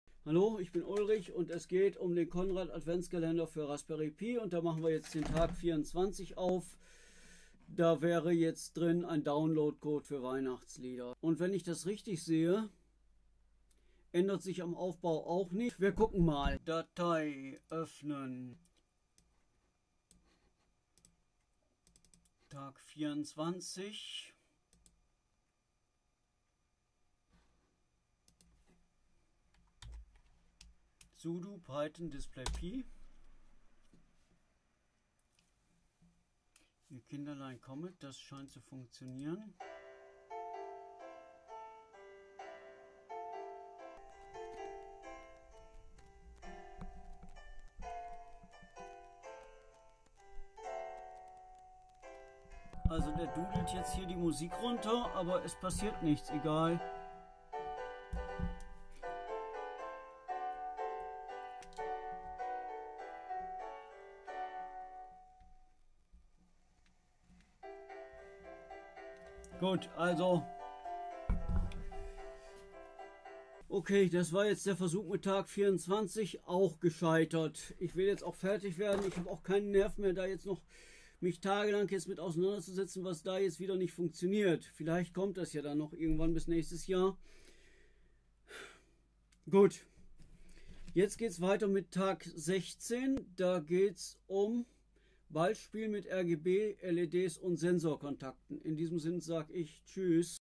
Tag 24; Conrad Adventskalender: Raspberry Pi 2016; Weihnachtsbaumschmuck mit MP3-Player
Aufbau des Experimentes vor laufender Kamera